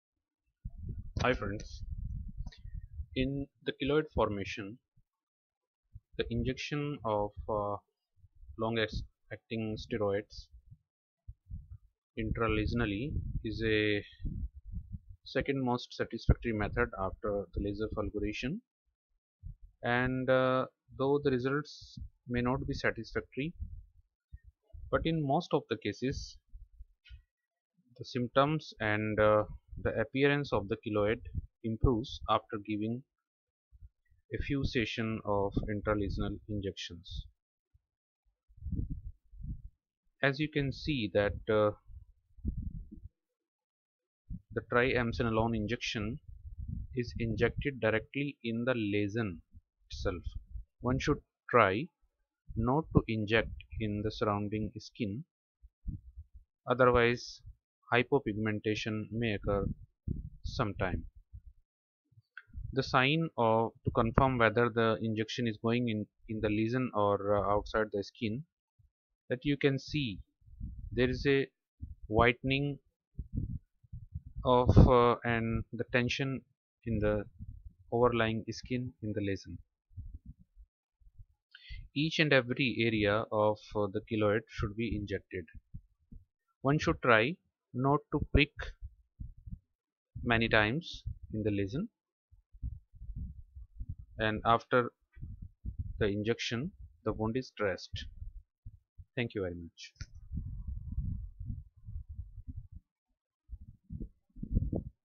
Keloid Neck - Intralesion Injection of Long Acting Steroid
Keloid is a very notorious disease to tackle and cure. Laser therapy is good. Intralesional injection is also very good.